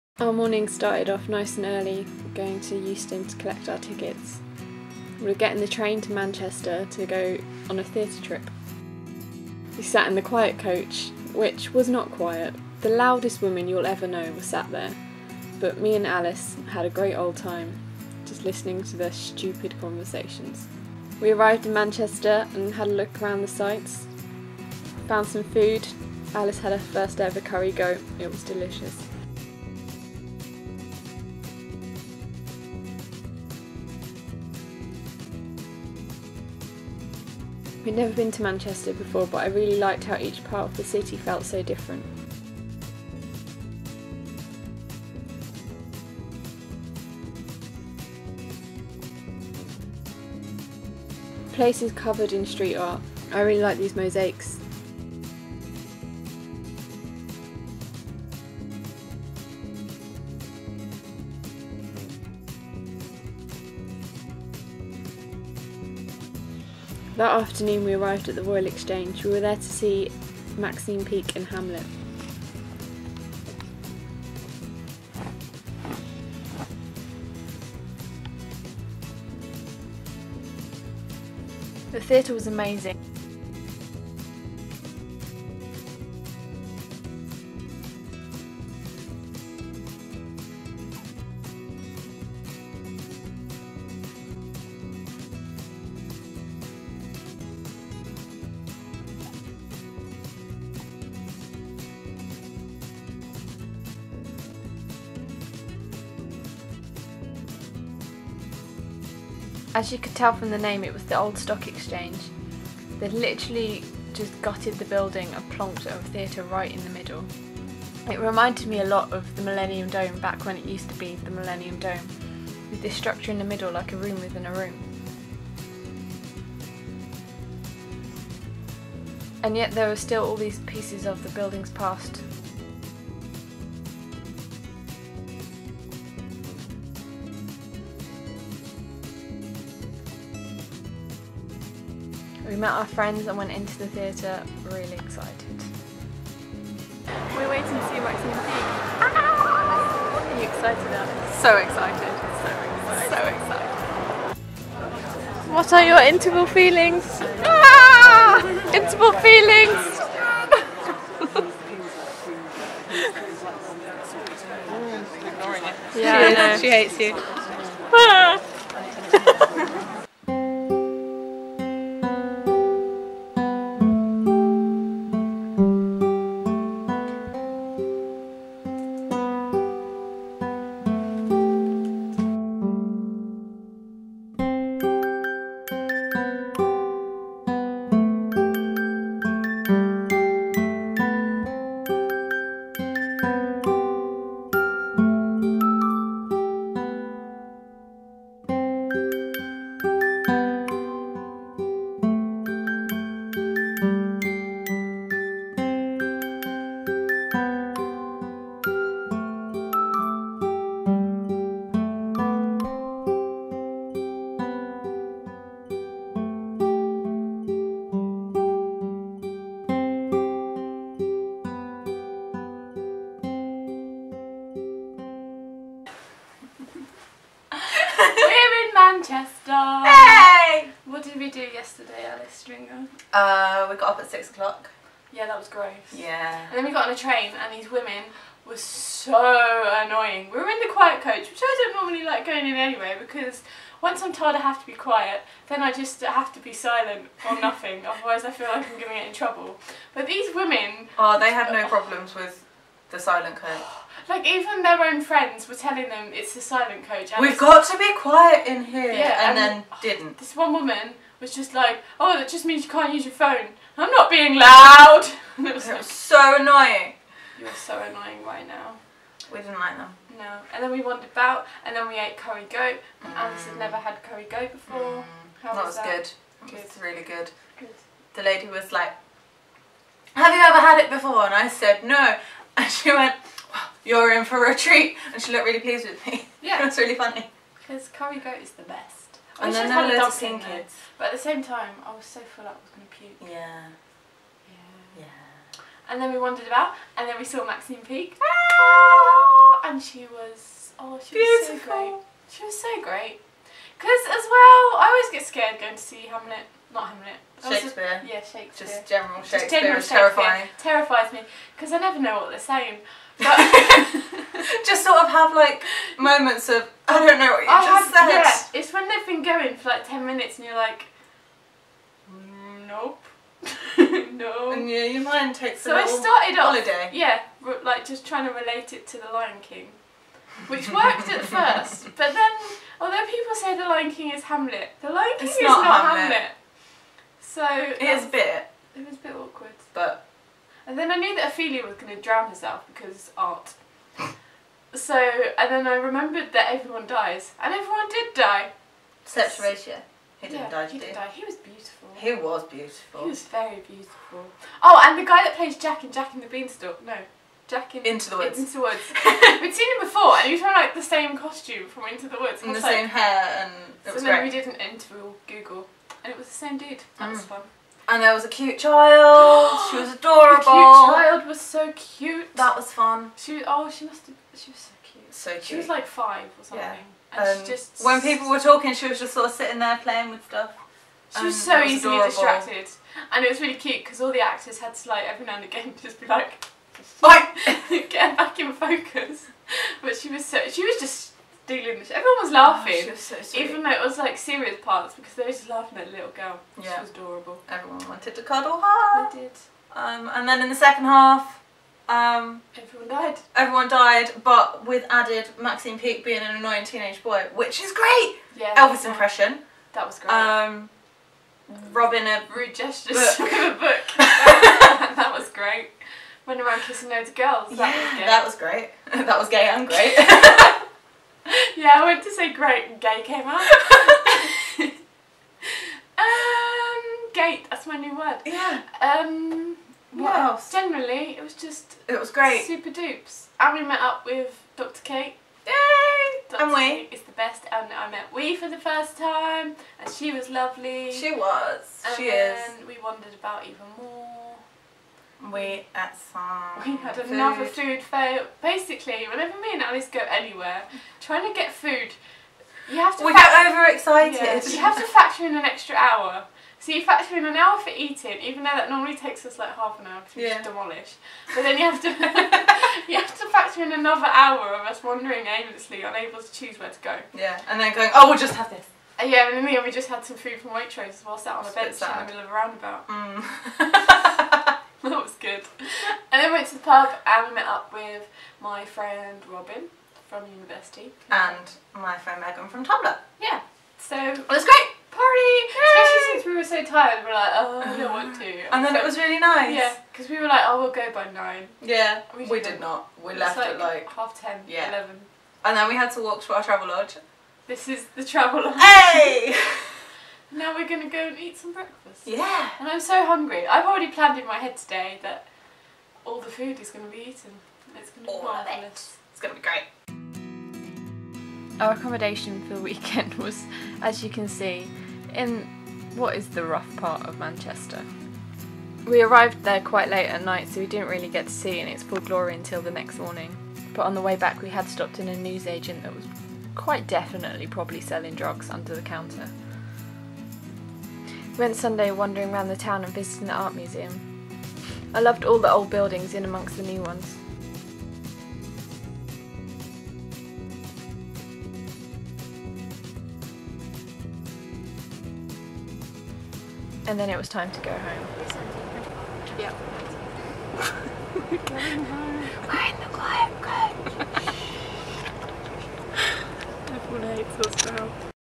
A Weekend in Manchester
Alice and I went to Manchester a few weeks ago to see Maxine Peake in Hamlet. This is the tale of that weekend.